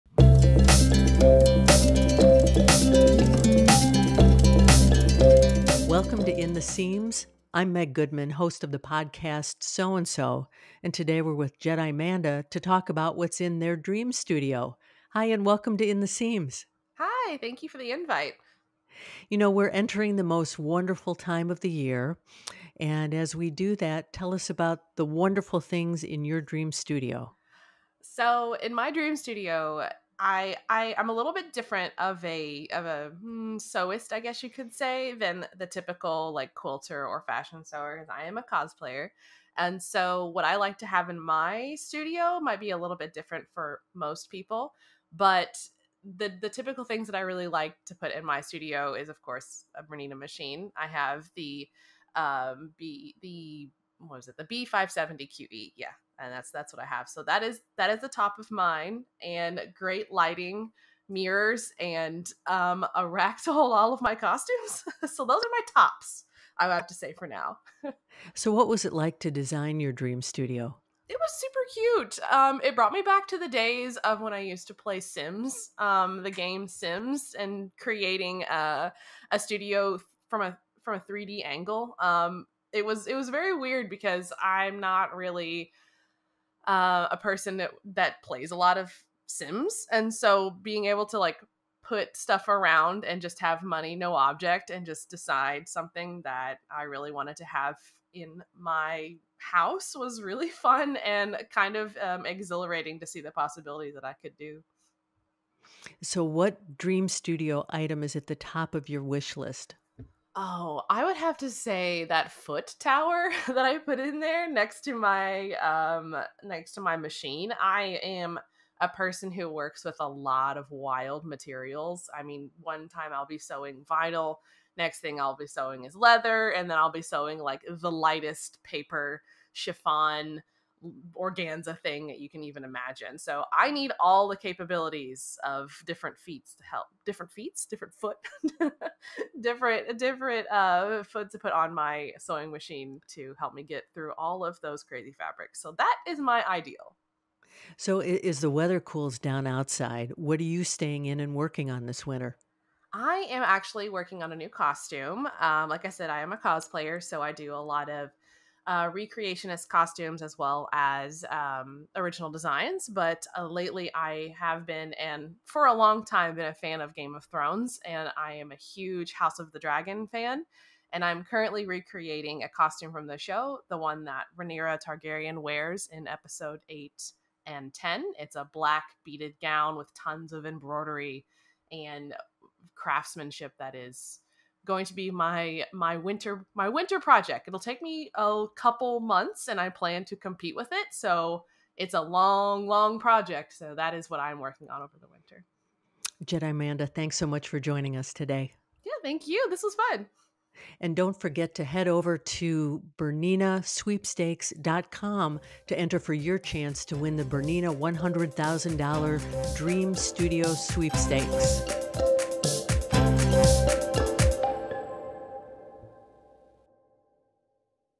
BERNINA $100K Dream Studio Sweepstakes | In The Seams | Jedimanda
Meg Goodman, host of Sew & So podcast, sat down with BERNINA Ambassador, Jedimanda to find out what it was like to create her BERNINA Dream Studio!

Design your dream sewing studio and enter for your chance to win one of the Dream Studio Grand Prize packages, as well as amazing monthly prizes

Check out the Official Sweepstakes Rules found on the Sweepstakes entry page for all of the giveaway details. The BERNINA $100K Dream Studio Sweepstakes ends July 27, 2023.